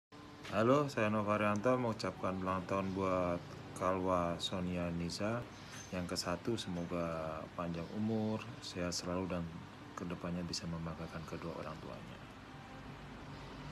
Nova Arianto I Eks Bek Persib Bandung & Timnas Indonesia
Nova Arianto Eks Bek Persib Bandung berikan ucapan Ulang Tahun ke 1 untuk Khalwa Shofiya Nizza

Hai teman teman dimana pun berada buat yg cinta sepakbola Indonesia, ada Moment bangga buat orang tua ketika mempunyai hobby dalam sepakbola sejatinya saya hanya penikmat sepakbola Indonesia khususnya, sebagai supporter abal abalan entah karbitan atau apalah itu namnya yg jelas ada rasa bangga aja ketika kenal dg mereka sebagai figur atau icon sepakbola saat memerikan ucapan ulang tahun jg luar biasa bahagia "Support Your Local Team" Sehat selalu Coach Nova.